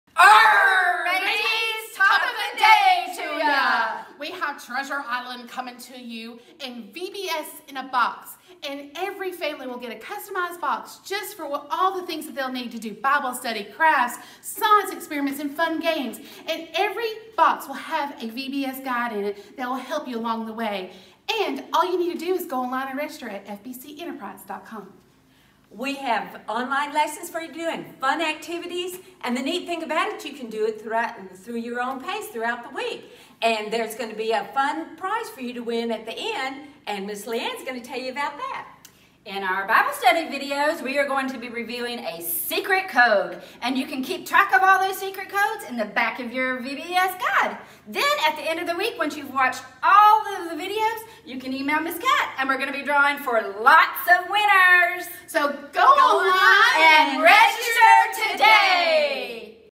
VBS in a Box! - Important Announcement
We are stilling having VBS this year! It will be different but our Children's Ministry is hard at work to ensure everyone has an amazing VBS experience.

Live on Facebook